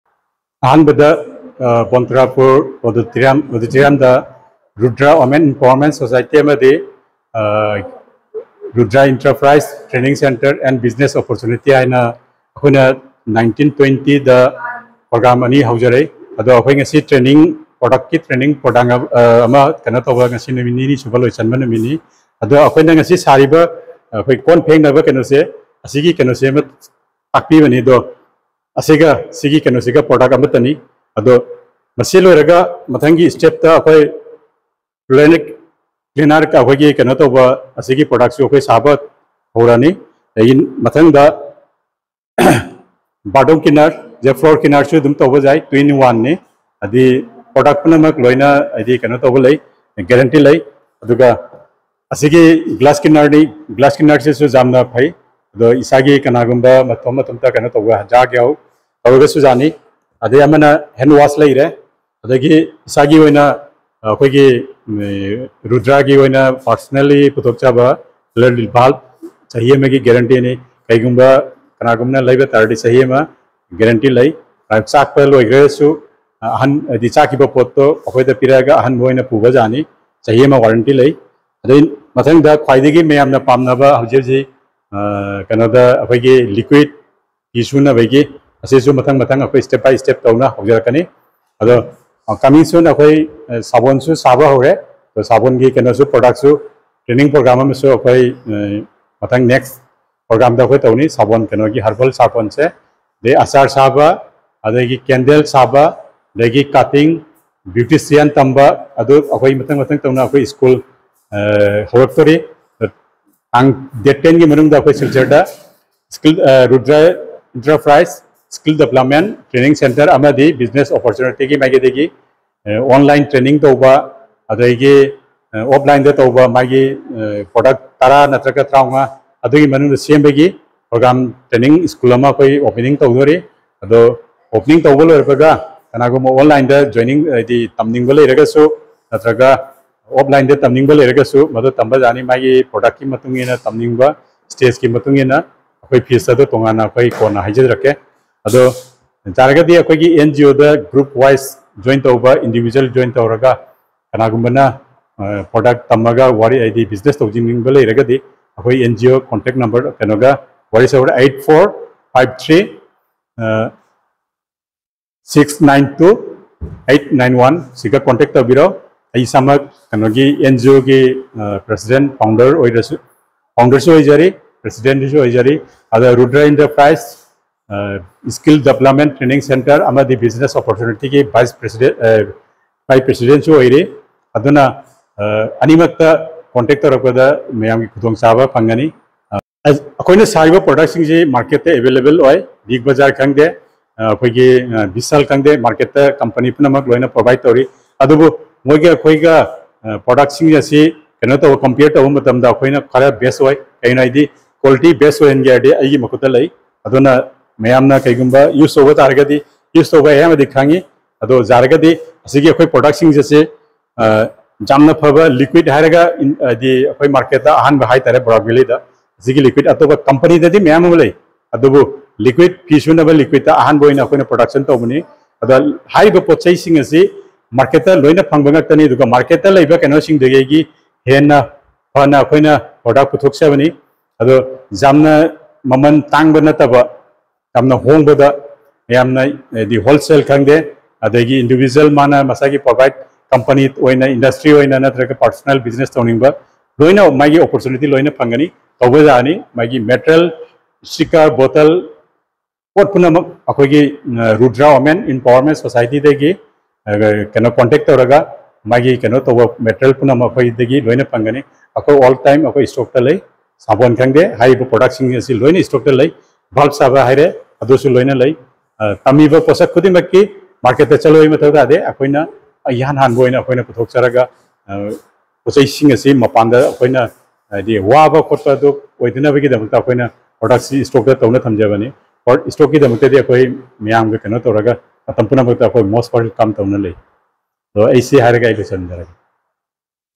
Ngairembam Manglemba na Society asina puthorakliba potlamsinggi maramda fongdorakhiba kaya taminnasi
Ngairembam Manglemba na Society asina puthorakliba potlamsinggi maramda fongdorakhiba kaya taminnasi || Numit nini chupna pangthokhiba Skill Development Training Program loisinba nunit || Bantarapur Auditorium Hall, Cachar, Assam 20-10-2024